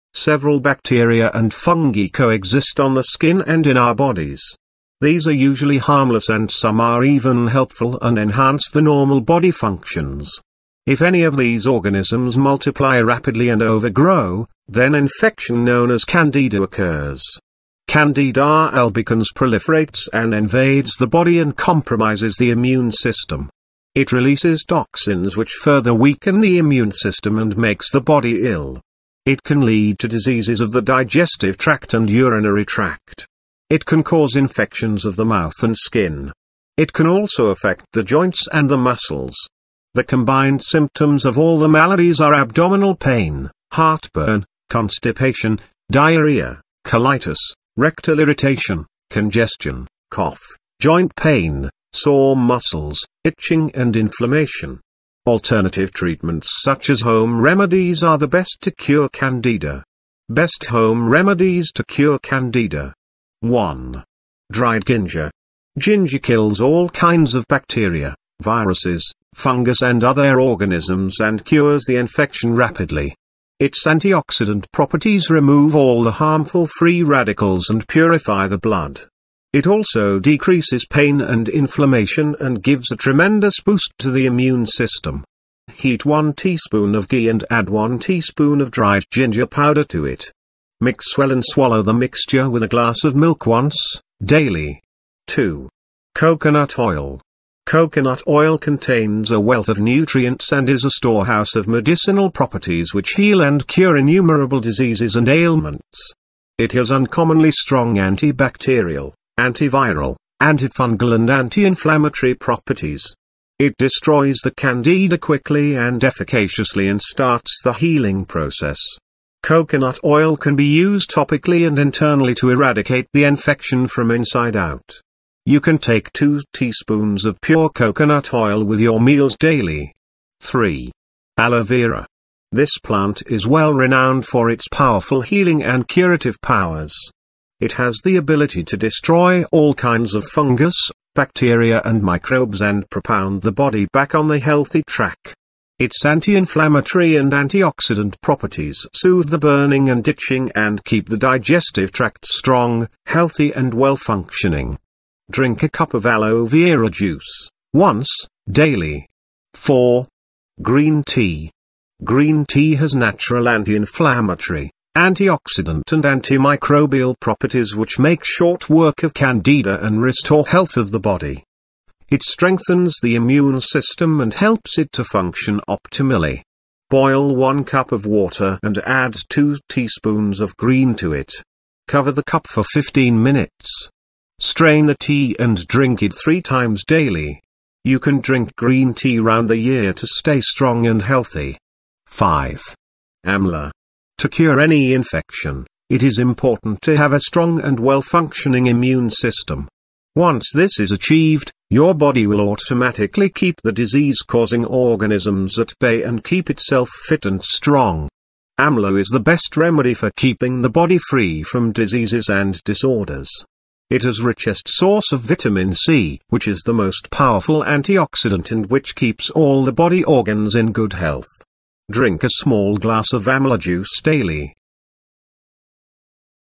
Home Remedies To Cure Candida
A few microscopic organisms and growths exist together on

the skin and in our bodies. These are typically safe and

some are even useful and upgrade the ordinary body

capacities. On the off chance that any of these creatures

duplicate quickly and congest, then disease known as

candida happens. Candida albicans multiplies and attacks

the body and bargains the invulnerable framework. It

discharges poisons which additionally debilitate the

invulnerable framework and makes the body sick.

It can prompt illnesses of the stomach related tract and

urinary tract. It can bring about contaminations of the

mouth and skin. It can likewise influence the joints and

the muscles. The consolidated manifestations of the

considerable number of diseases are stomach torment, acid

reflux, obstruction, loose bowels, colitis, rectal

aggravation, blockage, hack, joint agony, sore muscles,

tingling and irritation. Elective medicines, for example,

home cures are the best to cure candida.

Best Home Remedies To Cure Candida.

1.

Dried Ginger.

Ginger executes a wide range of microbes, infections,

growth and different life forms and cures the contamination

quickly. Its cancer prevention agent properties expel all

the unsafe free radicals and refine the blood. It likewise

diminishes torment and aggravation and gives a huge lift to

the resistant framework. Warm one teaspoon of ghee and

include one teaspoon of dried ginger powder to it. Blend

well and swallow the blend with a glass of drain once, day

by day.

2.

Coconut Oil.

Coconut oil contains an abundance of supplements and is a

storage facility of therapeutic properties which mend and

cure multitudinous illnesses and infirmities. It has

exceptionally solid antibacterial, antiviral, antifungal

and calming properties.

It decimates the candida rapidly and effectually and begins

the mending procedure. Coconut oil can be utilized

topically and inside to annihilate the disease from back to

front. You can take two teaspoons of unadulterated coconut

oil with your suppers day by day.

3.

Aloe Vera.

This plant is very much prestigious for its intense mending

and corrective forces. It can crush a wide range of

organism, microorganisms and microorganisms and propound

the body back on the sound track. Its calming and cell

reinforcement properties alleviate the consuming and

tingling and keep the stomach related tract solid, sound

and well working. Drink some aloe vera juice, once, day by

day.

4.

Green Tea.

Green tea has regular calming, cancer prevention agent and

antimicrobial properties which make short work of candida

and reestablish wellbeing of the body.

It fortifies the resistant framework and helps it to work

ideally. Bubble some water and add two teaspoons of green

to it. Cover the container for fifteen minutes. Strain the

tea and drink it three times day by day. You can drink

green tea round the year to remain solid and sound.

5.

Amla.

To cure any disease, it is imperative to have a solid and

well working insusceptible framework. When this is

accomplished, your body will consequently keep the malady

bringing on living beings under control and keep itself fit

and solid. Amla is the best solution for keeping the body

free from maladies and scatters. It is wealthiest

wellspring of vitamin C which is the most intense cell

reinforcement and which keeps all the body organs healthy.

Drink a little glass of amla squeeze day by day.